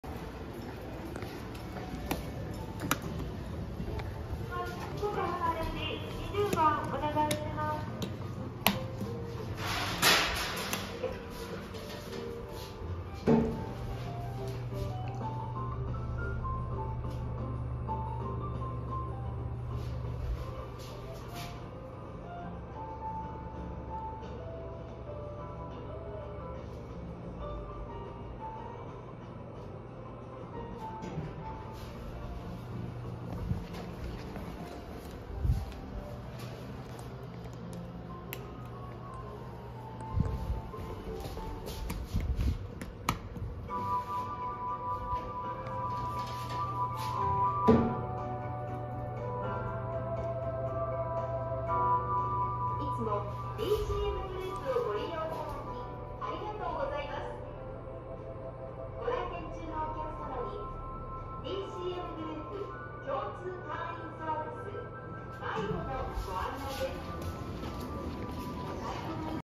DCM美沢店のエレベーター
機種　NEWルミナス
用途　乗用
定員　30名
積載　2000kg
停止階　1,2,R

時間19時に投稿したつもりが21時になってました